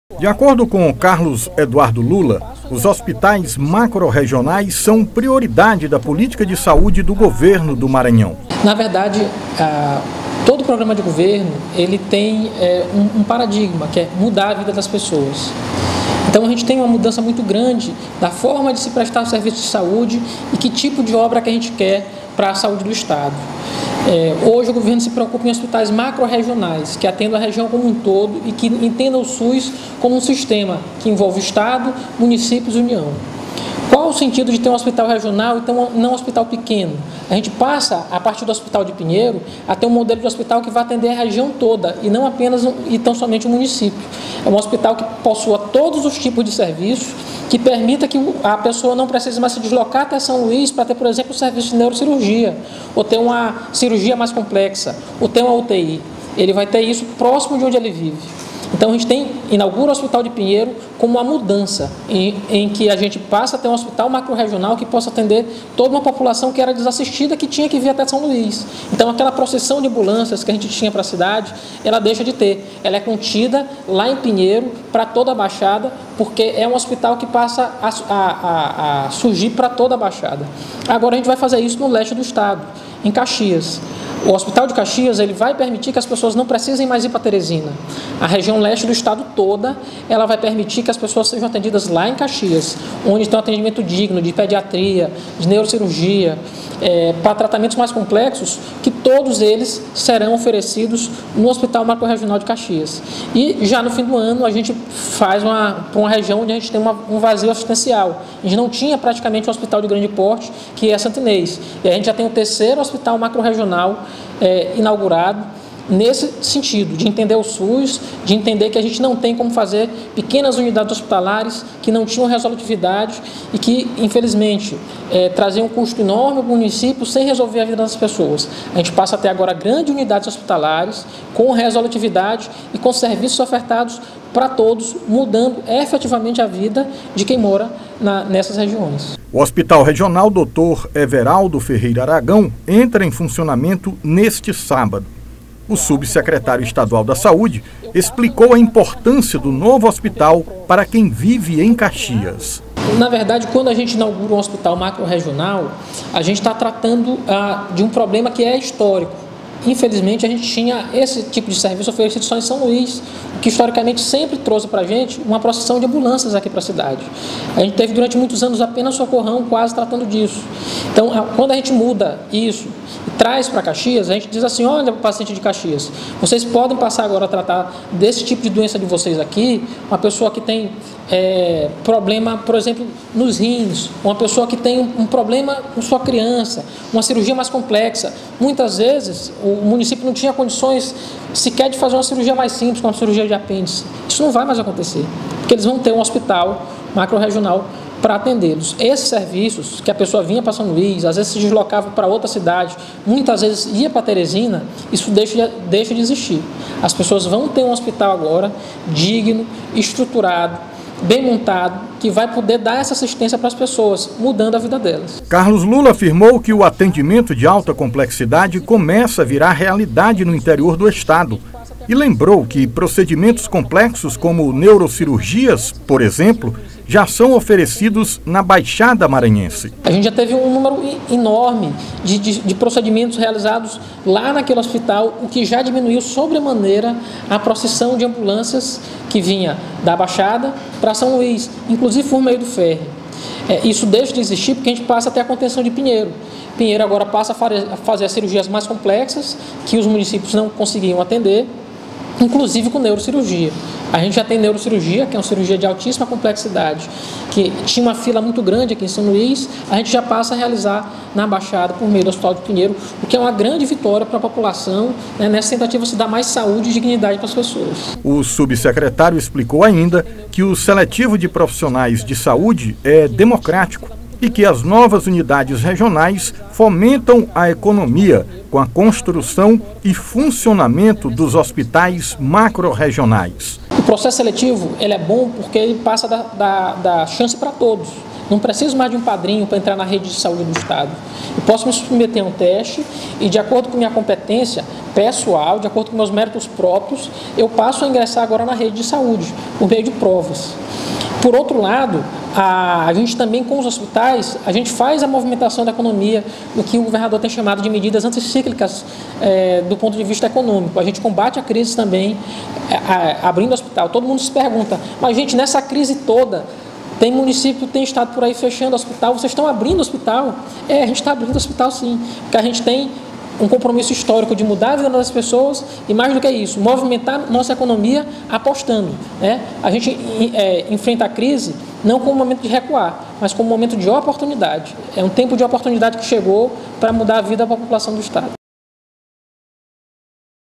Carlos Lula fala sobre hospital regional de Caxias
Sub-secretário Carlos Eduardo Lula